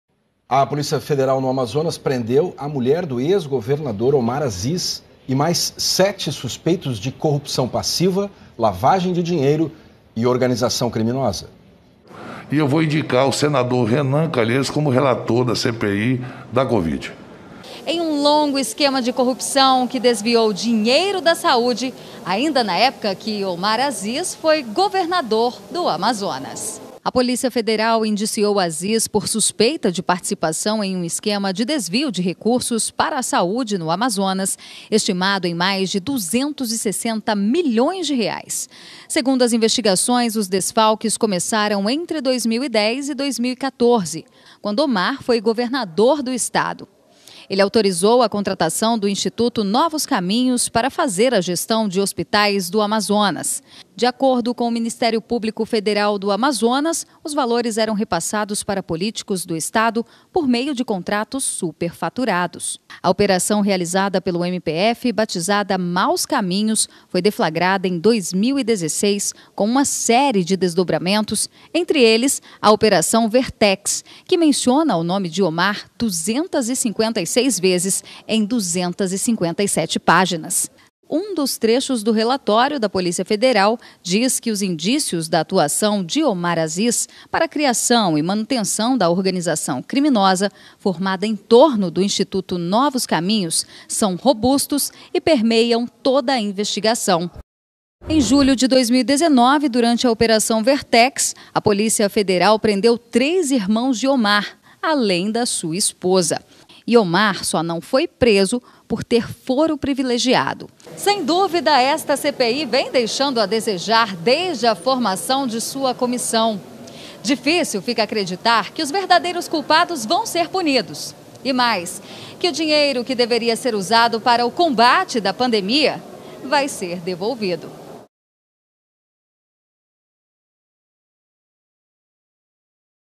Globo? denuncia Omar Asis presidente da CPI da covid por 260 Milhões desviado da saúde do Amazonas
Globo denuncia Omar Asis a CPI da falta de vergonha na cara tem como presidente um indiciado por desvio de R- 260 Milhões